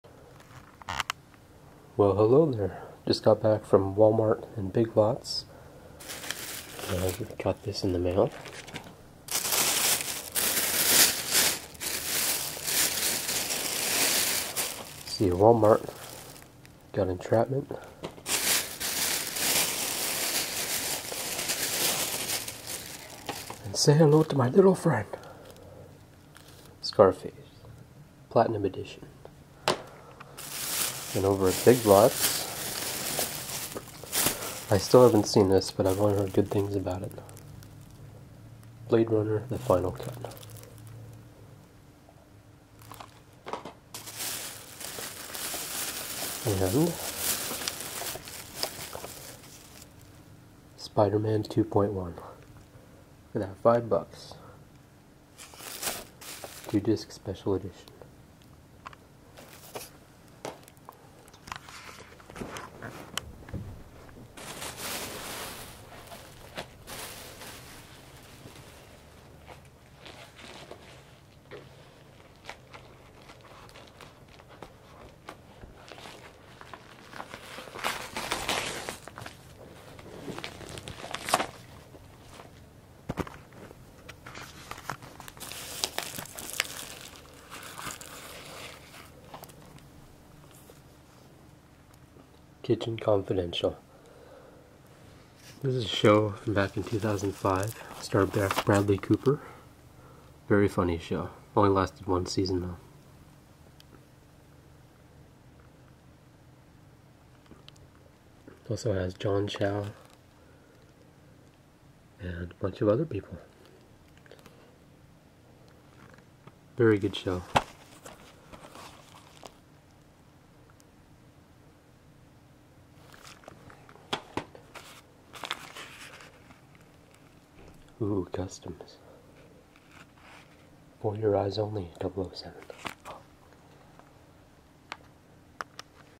NEW 2 DVD collection..
NEW to my DVD collection:
Blade Runner: Final Cut
Scarface: Platinum Edition
Entrapment
Kitchen Confidential
Spider-Man 2.1